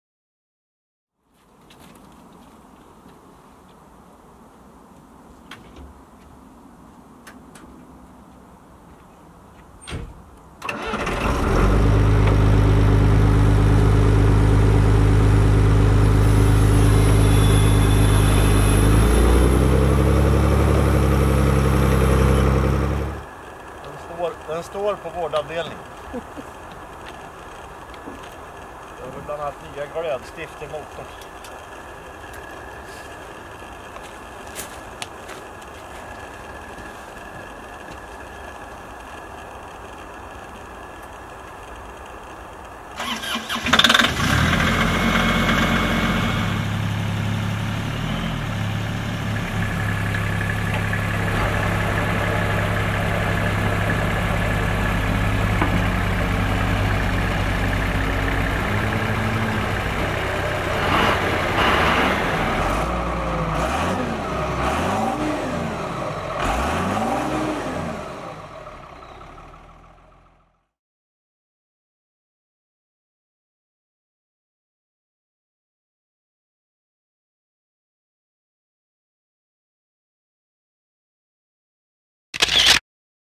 Maffigt motormuller –- hör till exempel Åkermangrävarens lite otippade läte.
Läs mer om Conny Kolsjö (länkar lite längre ned) och hur han fått sin Åkerman att ryta så "vadderat", läs också om varför han föredrar maskiner som har minst 25 år på nacken och bekanta dig med de gamla favoriterna (den "hårt trimmade" Hanixen, Åkermangrävaren som gick från C till D, med mera med mera!). Se länkarna nedan!